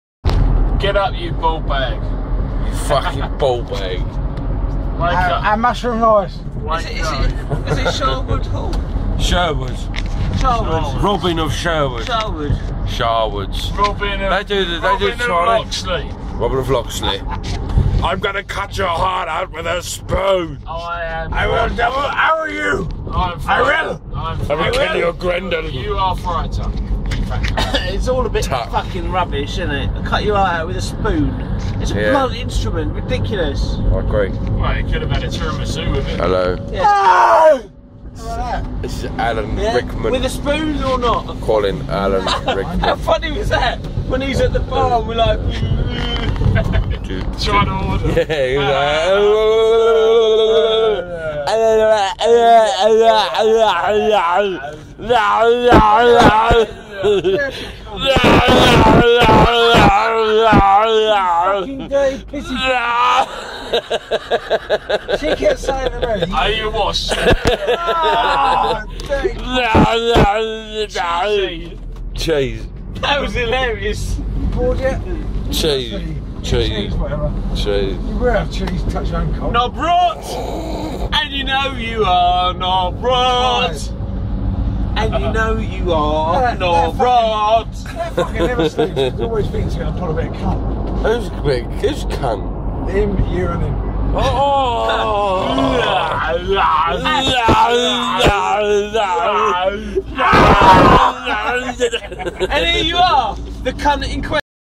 WSW11 - Cab Home Carnage
3 folded South East carnage mongerers on their way home after a night on the tiles of Oceana = random bollocks!
Highlights include;
"a... and mushroom rice!"
"I'm gonna cut your heart out with a spoon!!"
"Daaarlllarrrallalarlalarlalarrrllararal!" (zombie)